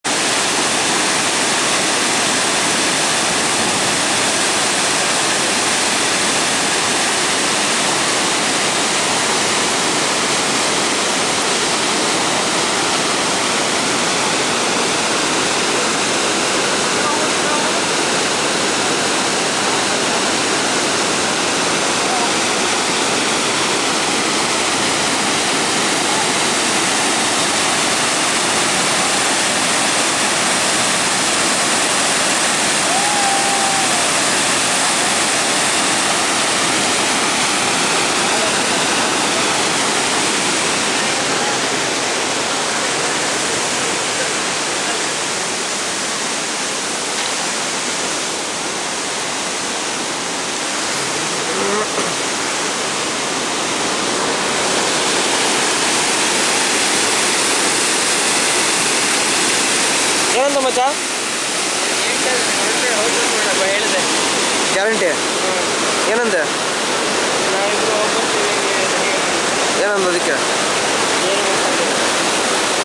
Dudhsagar Falls in Early Morning - Monsoon - july 2012 (Full HD)
Dudhsagar Falls in Early Morning - Monsoon - july 2012

Dudhsagar falls as viewed in early morning.
This video was captured at around 6 am.